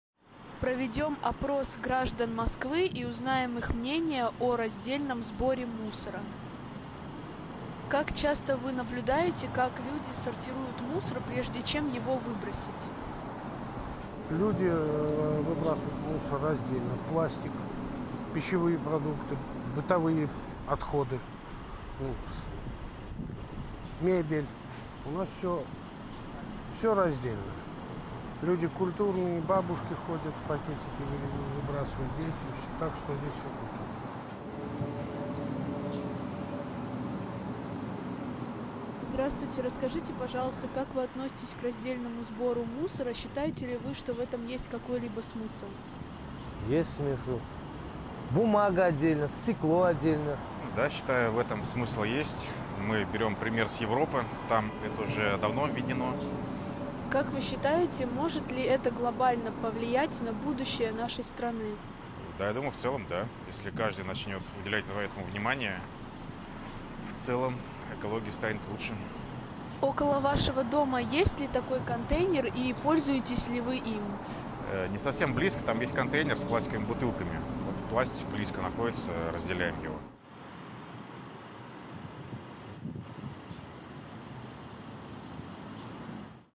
Социальное видео про раздельный мусор для Дипломной работы - Видеостудия VIP Production
Задача, снять видео "имитация работы студента" для дипломной работы по ТЗ тематичка "Новостной сюжет про раздельный сбор мусора" с озвучкой от заказчика.
Выполненные работы: Видео съемка 3 часа, Видеомонтаж,

Нужно снять или смонтировать подобный видеоролик?

Адрес — Москва, Ленинградское шоссе 21, 4А (м.Войковская)
Круглосуточно Без Выходных